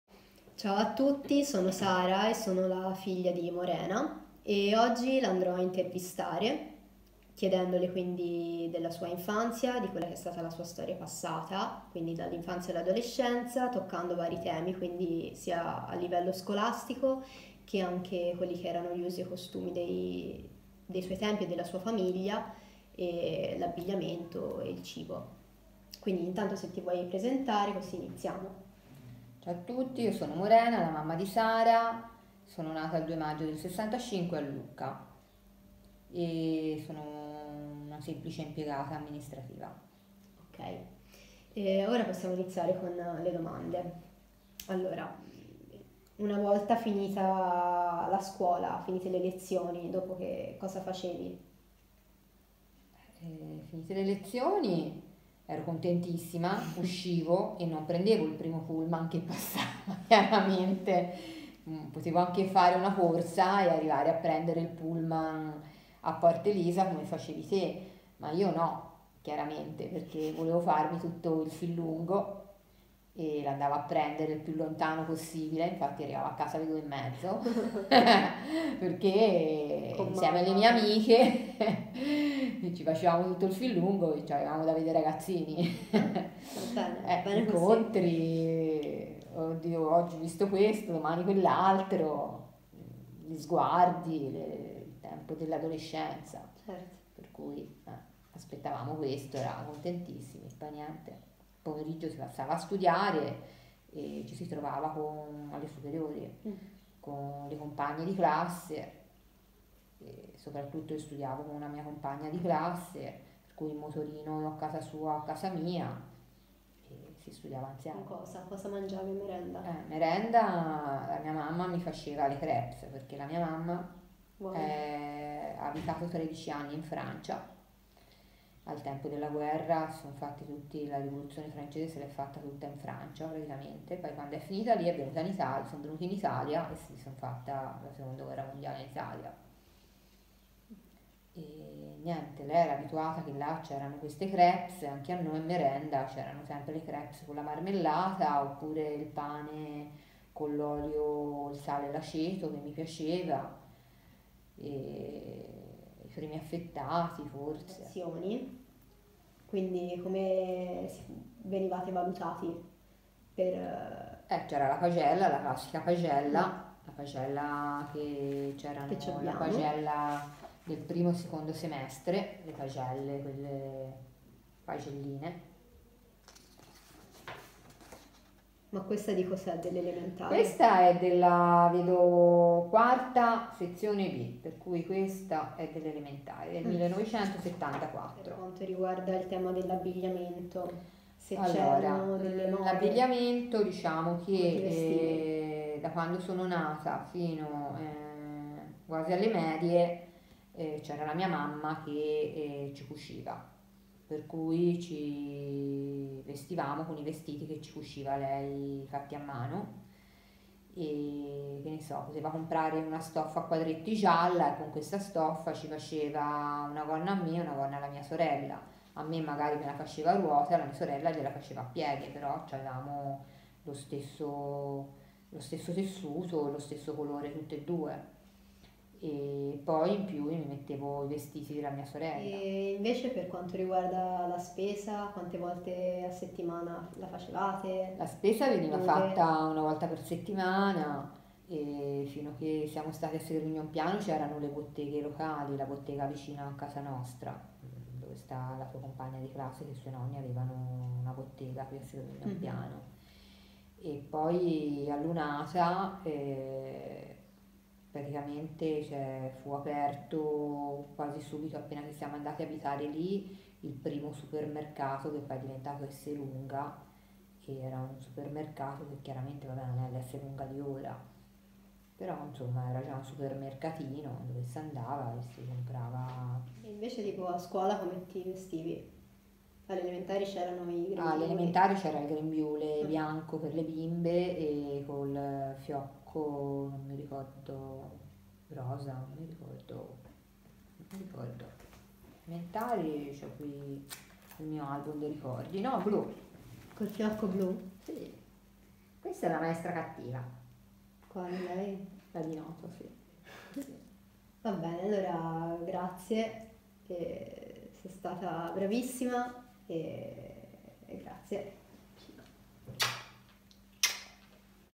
Trailer intervista a Morena Franchi
Testimonianza raccolta da Sara Bertini, il giorno 09/05/2020, a Segromigno in Piano (LU). Attività svolta all'interno del Progetto di ricerca di rilevante interesse nazionale, bando 2017, School Memories between Social Perception and Collective Representation (Italy, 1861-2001).